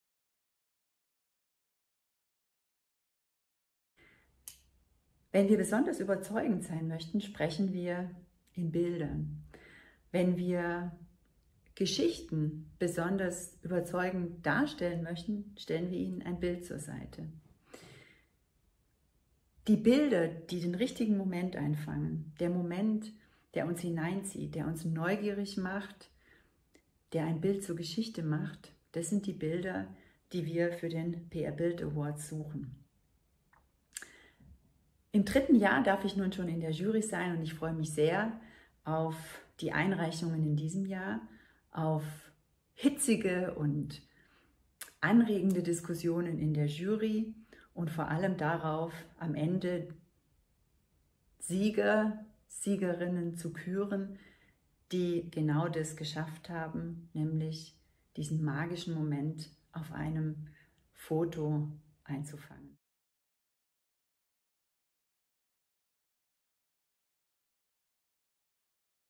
PR-Bild Award 2021: Meet Mirjam Berle
prba21-Juryvorstellung: Mirjam Berle ist bereits zum 3. Mal bei der Jurysitzung dabei, allerdings zum 1. Mal als Vorsitzende. Worauf sie sich vor der Sitzung am meisten gefreut hat, erfahrt ihr im Video! 🤩